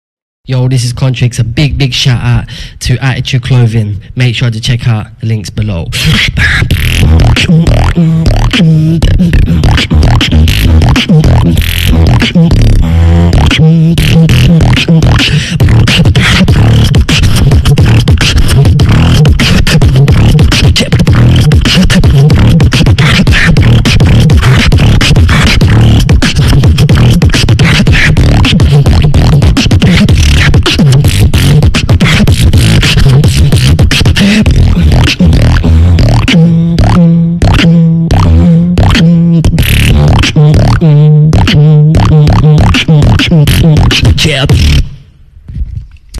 Attiitude Beatboxer | Contrix Beatbox
Sick beatz on the mic.
2 x UK Beatbox Champion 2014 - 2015 with Attiitude.